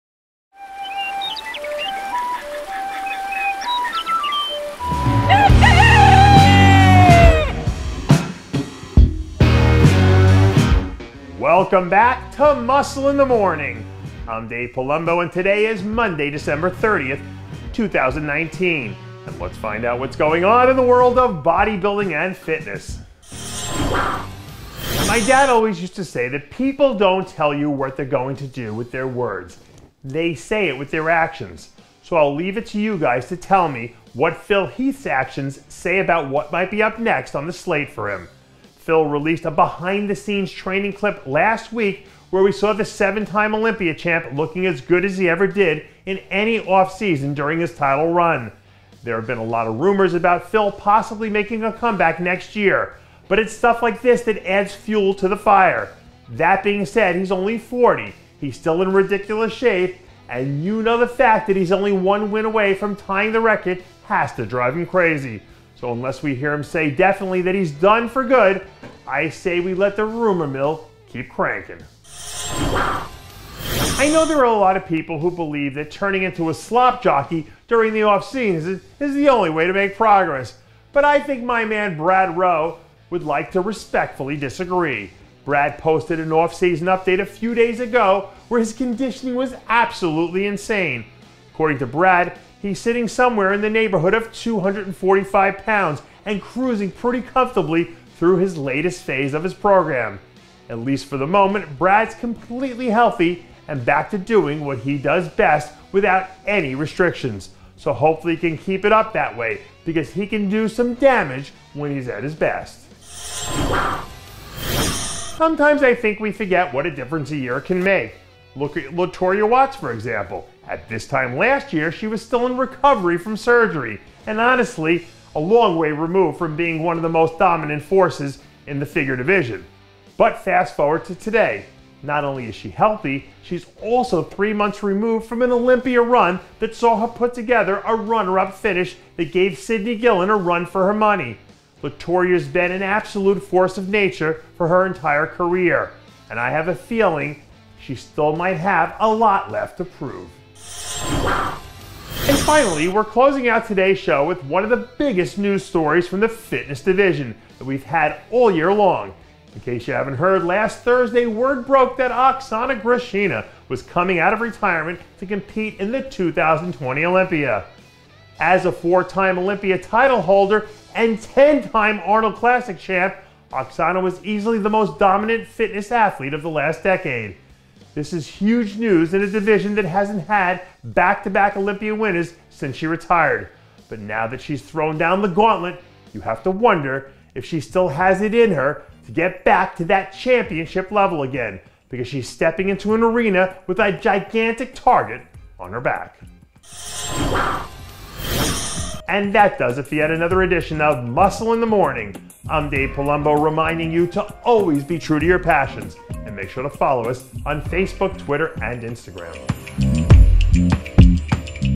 PHIL HEATH LATEST RUMORS? Muscle in the Morning (12/30/19)
On Today's Muscle in the Morning:

Phil Heath
Brad Rowe
Latorya Watts
Oksana Grishina